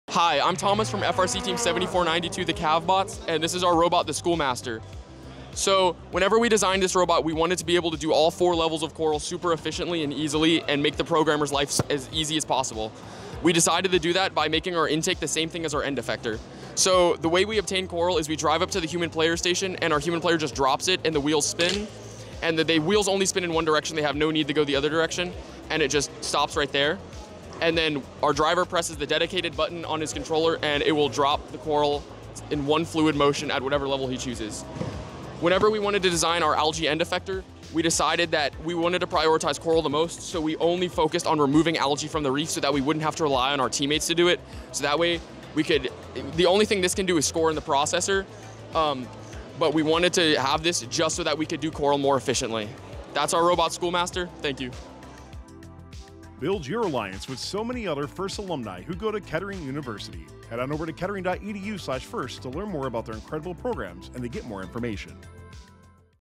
'Schoolmaster' Robot | 7492 CavBots | Pit Stop
7492 CavBots demonstrates their REEFSCAPE robot 'Schoolmaster' and the versatility it has for scoring.

This video is supported by Kettering University: Accepted @KetteringUniversity1919 students can receive a FIRST/VEX Scholarship for up to $5000/year. to learn more.